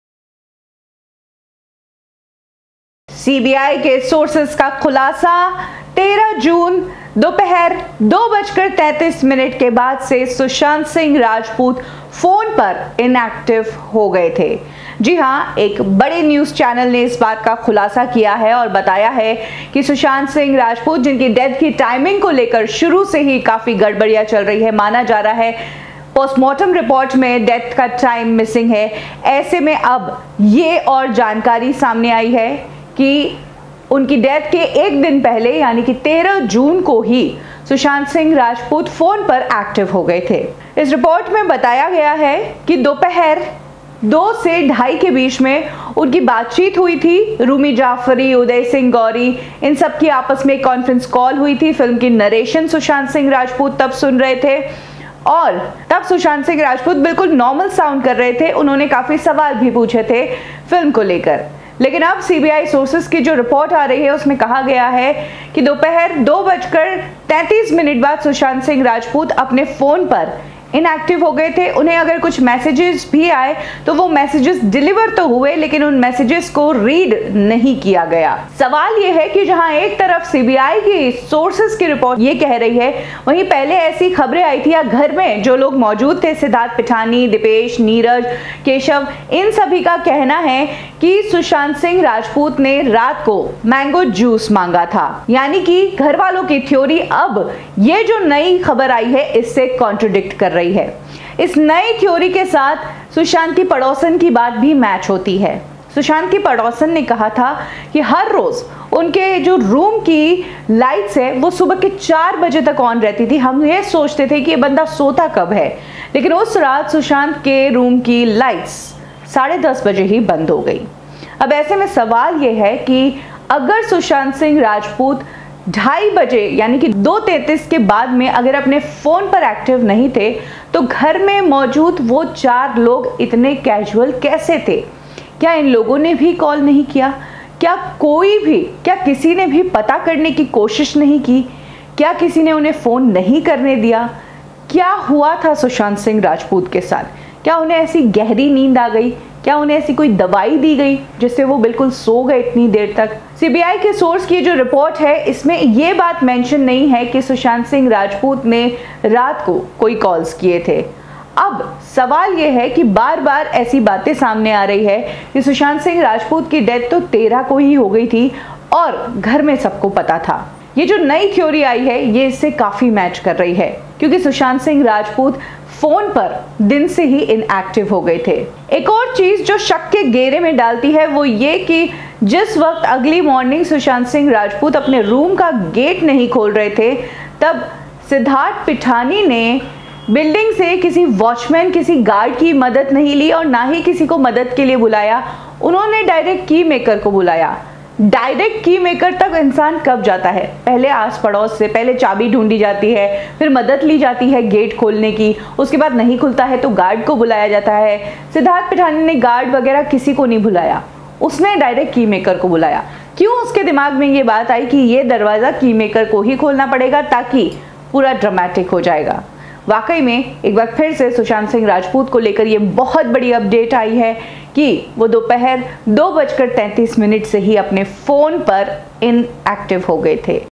बड़ा खुलासा | Sushant 13 June को 2 बजे से Inactive हो गये थे अपने Phone पे | तो कौन पढ़ रहा था Msg ?
Sushant Singh 13 June को 2 बजे से Inactive हो गये थे अपने Phone पे | तो कौन पढ़ रहा था Msg और किसके हाथ में था Phone |